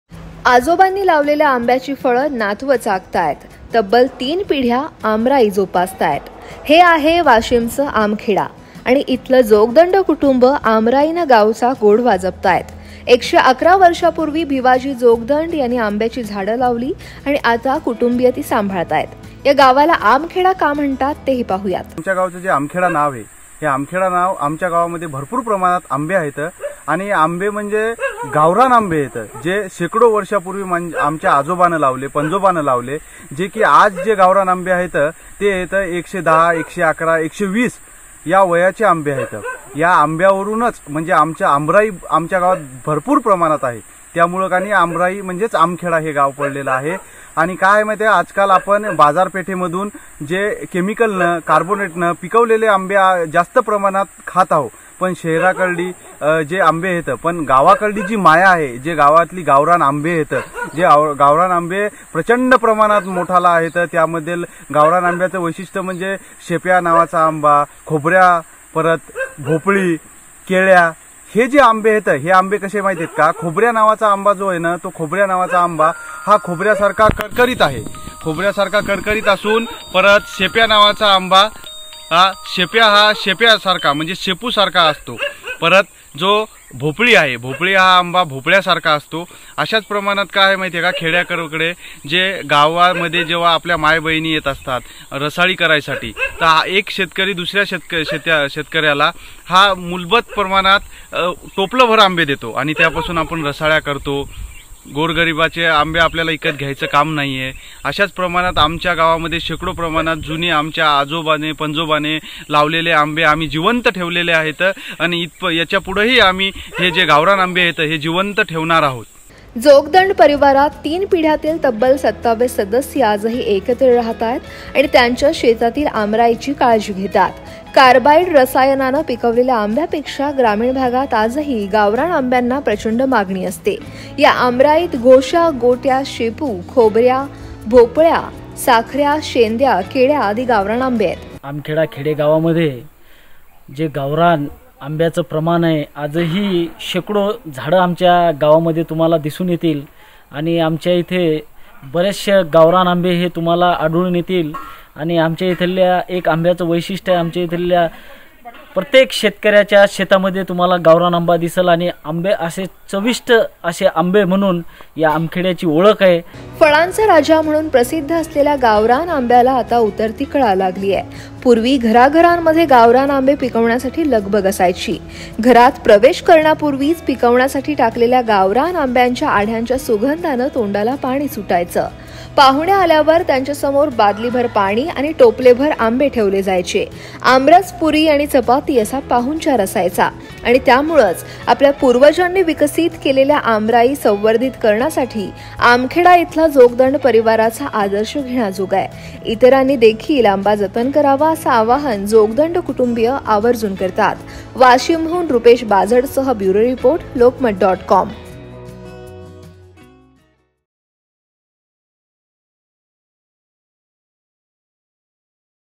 वाशिममध्ये आजोबांनी झाडं लावली, नातवं आंबे खातायत, तिसरी पिढी जपतेय बाग | Washim Farmers Story | AC3
वाशिममध्ये आजोबांनी झाडं लावली, नातवं आंबे खातायत, तिसरी पिढी जपतेय बाग | Washim Mango Farmers Success Story | AC3